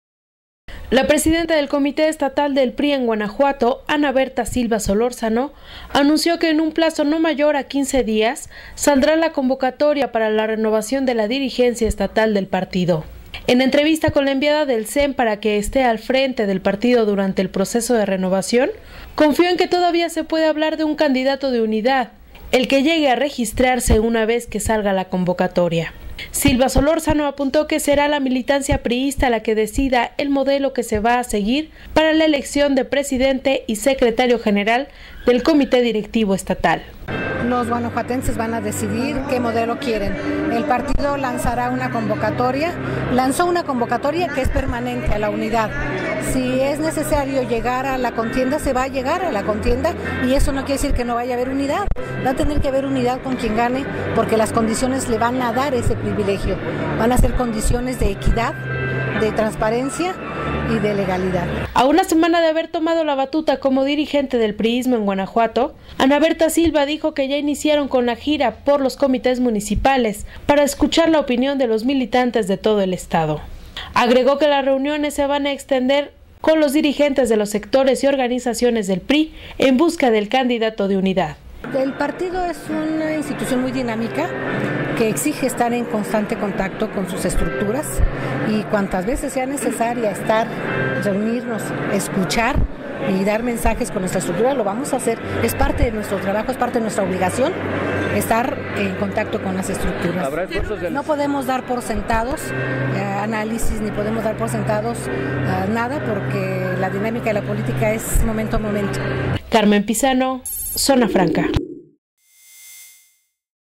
En dos semanas saldrá la convocatoria para renovar la dirigencia estatal del PRI
Guanajuato, Gto. La presidenta del Comité Estatal del PRI en Guanajuato, Ana Bertha Silva Solórzano, anunció que en un plazo no mayor a 15 días saldrá la convocatoria para la renovación de la dirigencia estatal del partido.